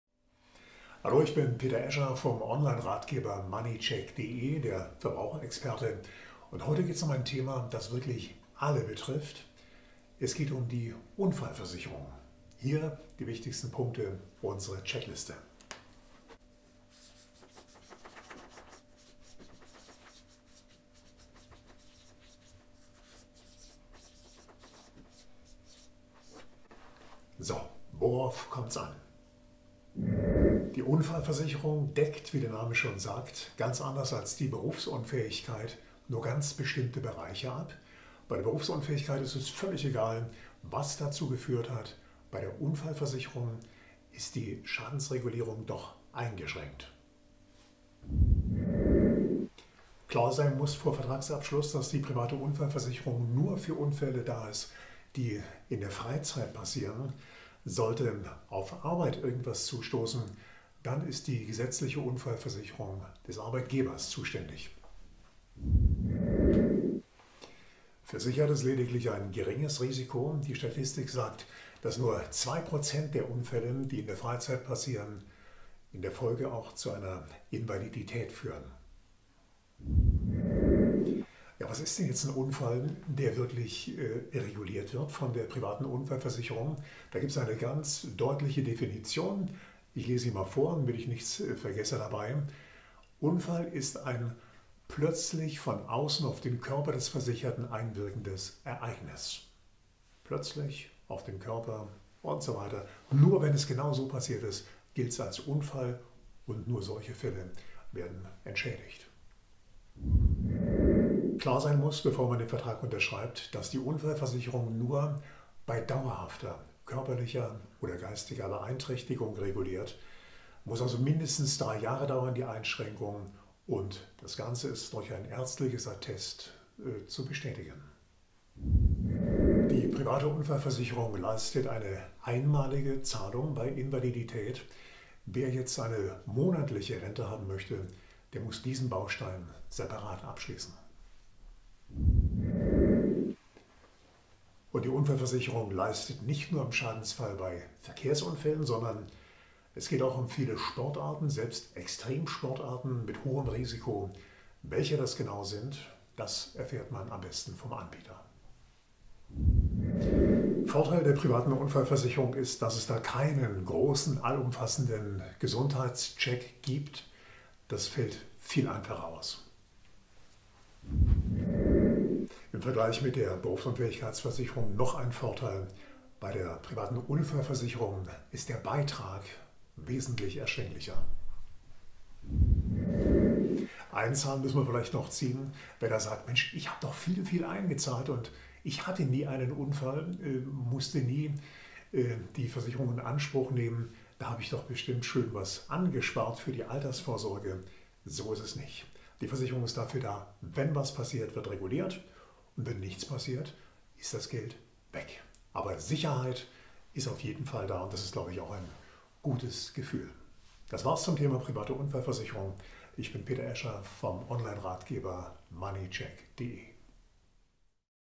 PRIVATE UNFALLVERSICHERUNG - die Checkliste
Wann springt die private Unfallversicherung ein? Wie ist ein Unfall definiert? Was unterscheidet sie von der Berufsunfähigkeitsversicherung?